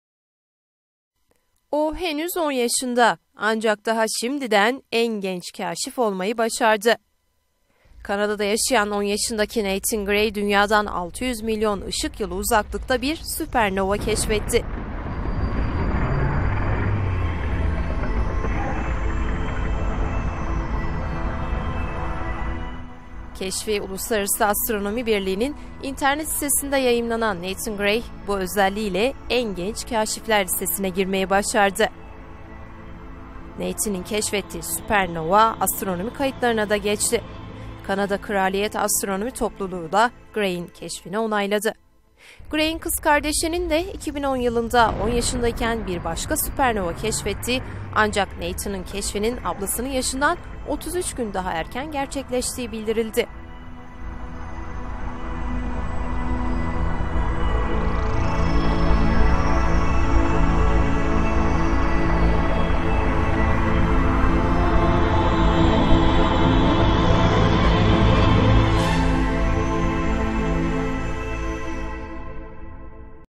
10 yaşında süpernova keşfetti!
p
 Kanada'da yaşayan 10 yaşındaki Nathan Gray, yaklaşık 600 milyon ışık yılı uzaklıkta bir süpernova keşfetti. Gray, süpernovayı, ülkenin Nova Scotia eyaletindeki Abbey Ridge Gözlemevi tarafından çekilen görüntüleri tararken farketti./p
p
p
10 yaşında süpernova keşfetti!
10 yaşında süpernova keşfetti!
10 yaşında süpernova keşfetti!